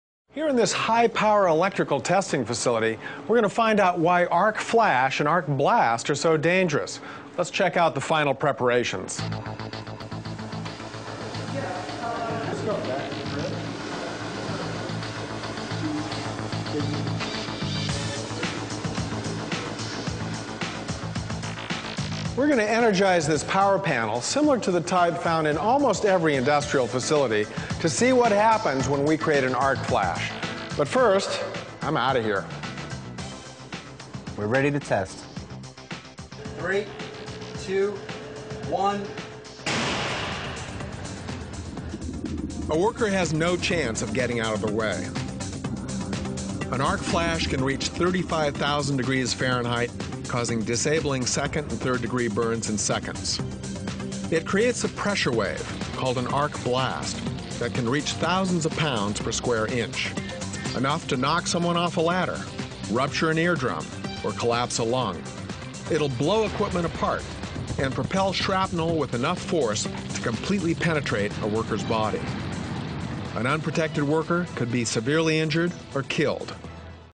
Electrical Arc Flash Demonstration
Don't be a dummy; never work on live equipment without using full protective gear. A micro second is all it takes to ruin your life (if you survive)